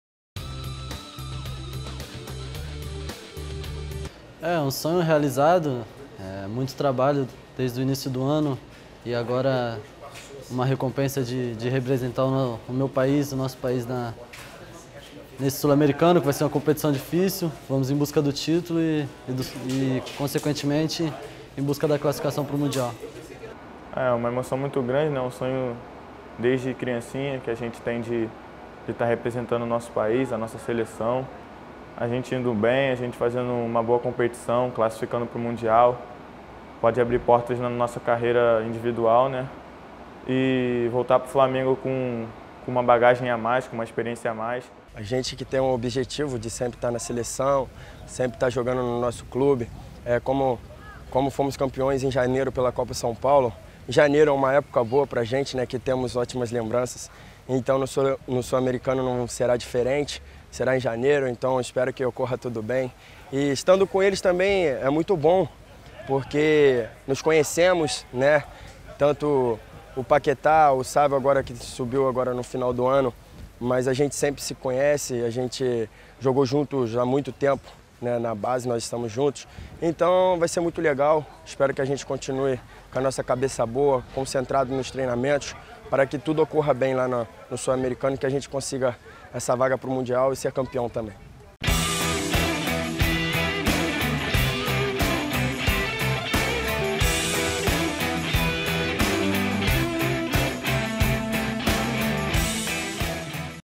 Matheus Sávio, Lucas Paquetá e Felipe Vizeu - Sul-Americano Sub-20
Matheus Sávio, Lucas Paquetá e Felipe Vizeu irão defender a Seleção Brasileira Sub-20 no Sul-Americano no Equador.